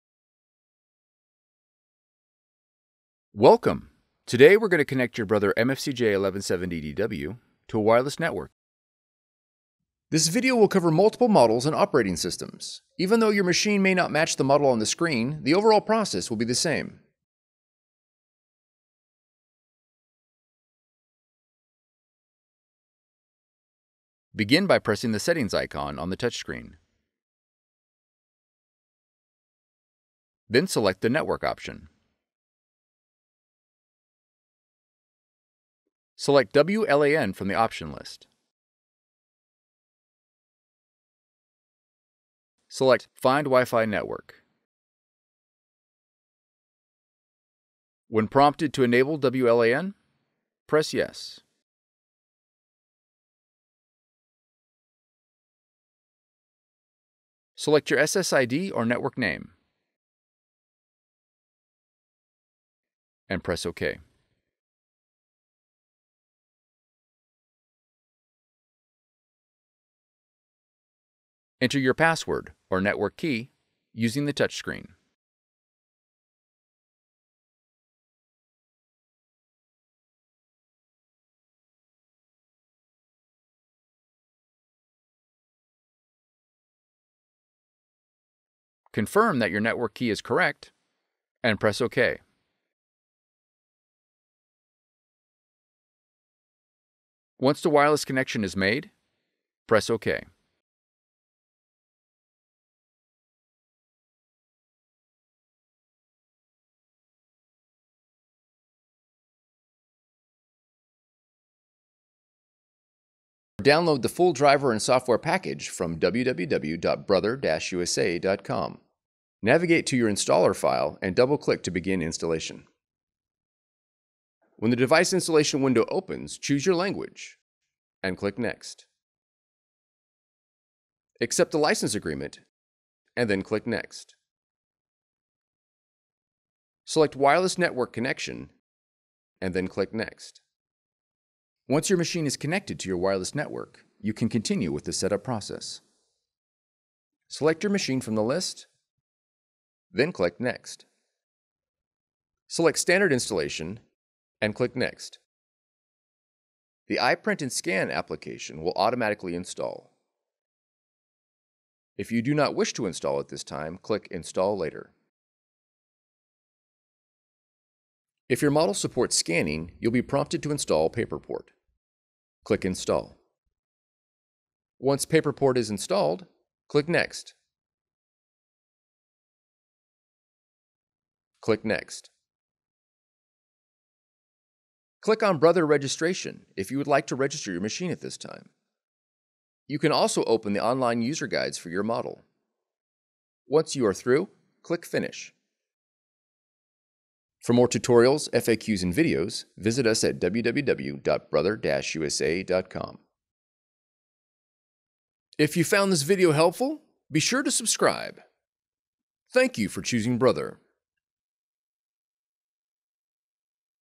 MFCJ1170DW wireless setup from control panel - Windows
Learn how to set up your Brother MFC-J1170DW inkjet printer on a wireless network in this video tutorial. This method will use your machine’s control panel to establish the wireless connection.

Models covered:
Brother MFCJ1170DW

Chapters:
Intro 00:00
Models covered 00:10
Select network name 01:00
Enter network password 01:10
Download the software 01:50
Running the Installer software 02:00
Install iPrint&Scan 2:30
PaperPort Install 02:50
User’s Guide and Registration 03:00

Transcript:
Welcome, today we’re going to earn how to set up your Brother MFCJ1170DW to a wireless network using the machine control panel

Begin by pressing the settings icon on the touchscreen.

Then select the Network option

Select “WLAN” from the option list (Enable WLAN screen)

Select “Find Wi-Fi Network”

Enter your Network Key, using the touchscreen.

Download the full driver and software package Navigate to your installer file and double-click to begin installation.

Once your machine is connected to your wireless network you can continue with the setup process

Select "Wireless network connection (Wi-Fi)" and click "Next"

The iPint&Scan application will automatically install. If you do not wish to install at this time click “Install Later”

If your model supports scanning you will be prompted to install PaperPort.

Click "Install."

Click "Next"

Click on Brother registration to register your machine at this time. You can also open the online user guides for your model
Once you are through, click "Finish."

Customers also searched:
Brother MFC J1170DW wireless control panel Windows
Brother MFC J1170DW wireless setup control panel Windows